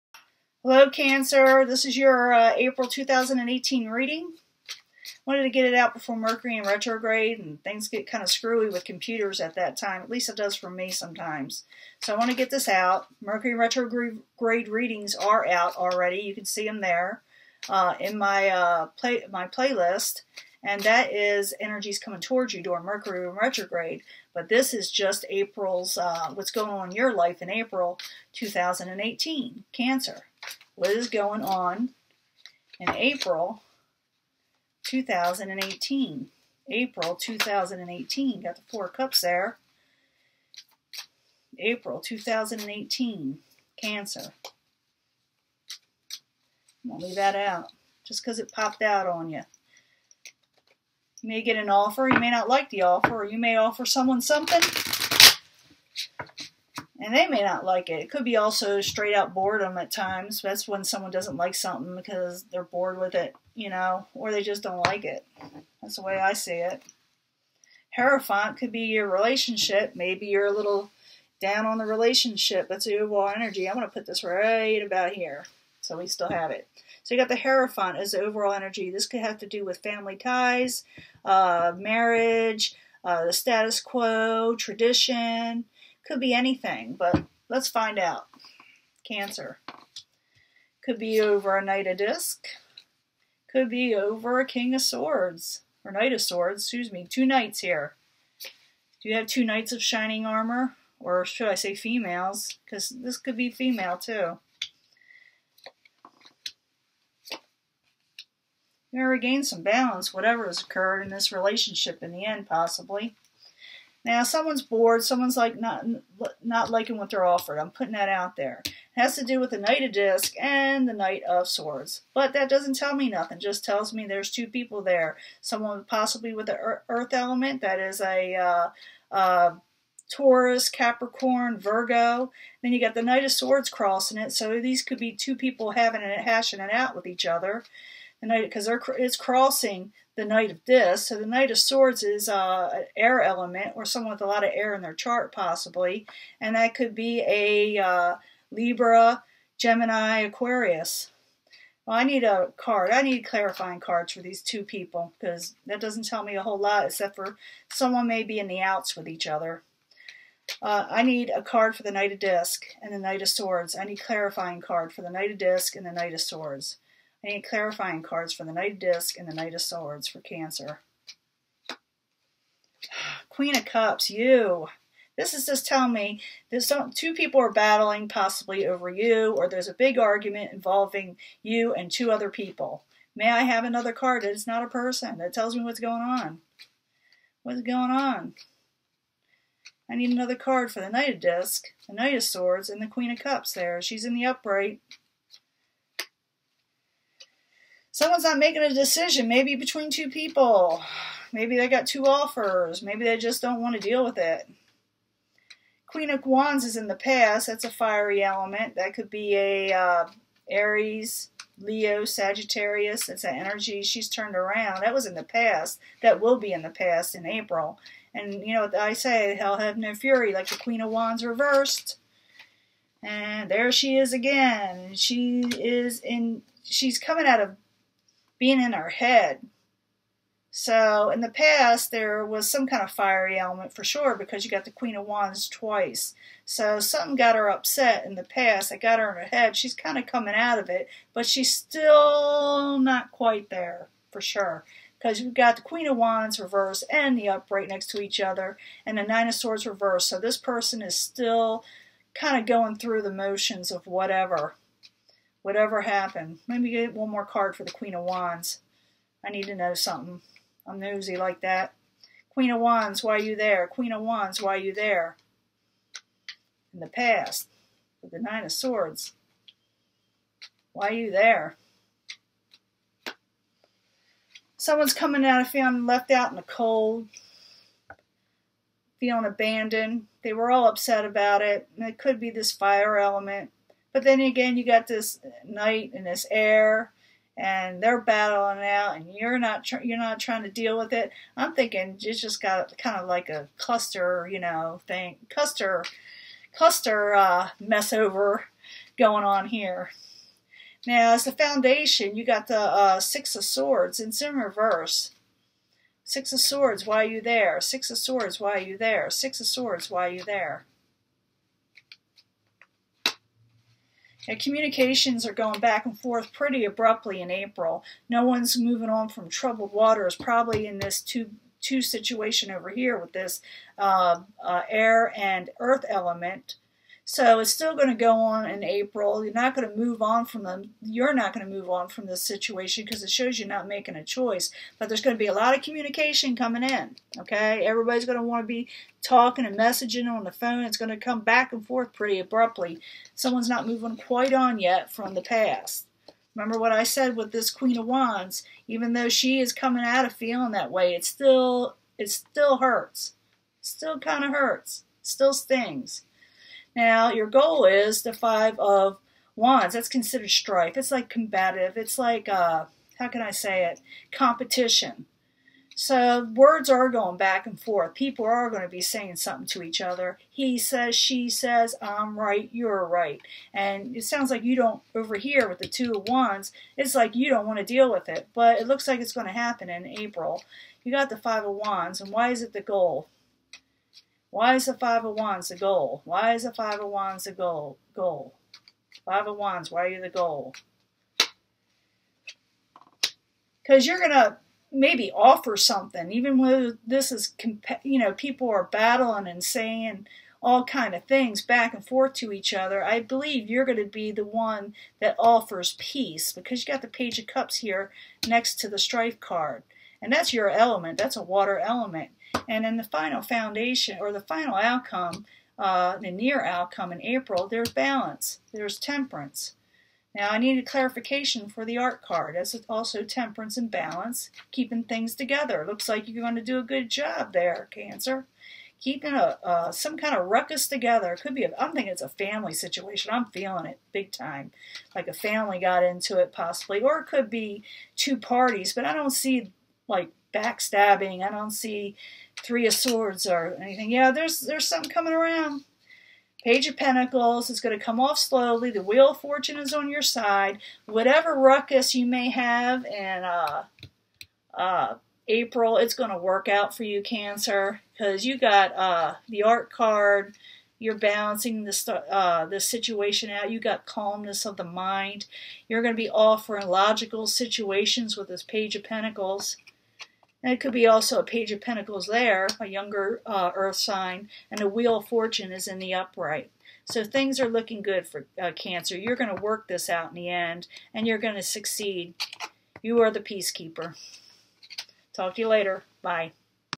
Cancer, You Are The Peacekeeper! April 2018
Cancer, You Are The Peacekeeper! April 2018, Tarot Reading!

Hello Everyone,

I try to show the cards for those who are hearing impaired.

(Spark Portal Generation, Slo-Mo Explosion 3)

Tarot Decks I Use:
-- Universal Waite Pocket Edition by Pamela Colman Smith & Mary Hanson-Roberts
-- Thoth by A. Crowley